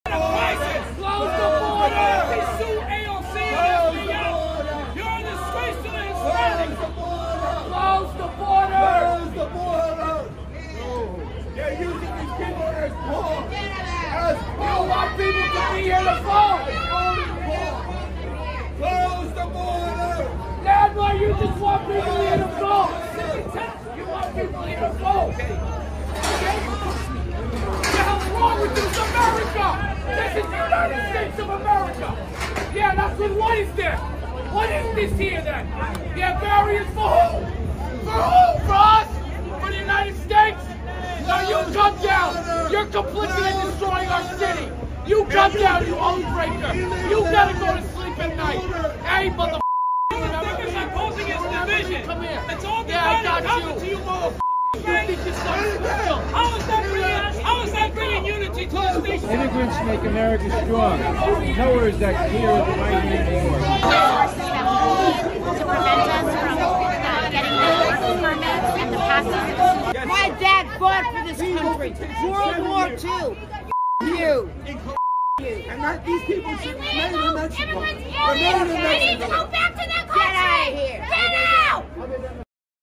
Chaos erupts as furious protesters heckle AOC during press conference for NYC’s migrant crisis
Furious protesters screaming “Close the border” heckled Rep. Alexandria Ocasio-Cortez, Rep. Jerry Nadler and other Democrats Friday as they were holding a press conference on the Big Apple’s escalating migrant crisis.

The New York pols were drowned out by the angry screams outside the Roosevelt Hotel mega-shelter in Midtown on Friday morning.

“Send them back,” one man shouted as the chaotic scenes unfolded outside the city’s main migrant intake site.